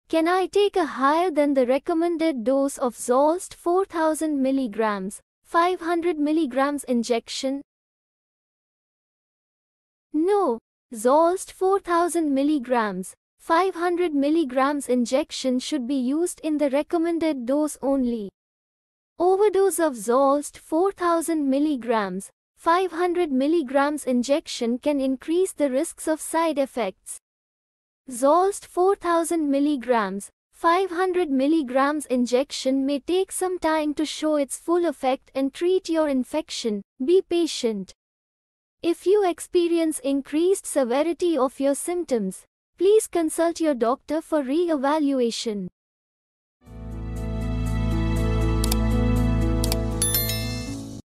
Can I take a higher than the recommended dose of Zolsed 4000 mg/500 mg Injection?
No, Zolsed 4000 mg/500 mg Injection should be used in the recommended dose only. Overdose of Zolsed 4000 mg/500 mg Injection can increase the risks of side effects. Zolsed 4000 mg/500 mg Injection may take some time to show its full effect and treat your infection, be patient. If you experience increased severity of your symptoms, please consult your doctor for re-evaluation.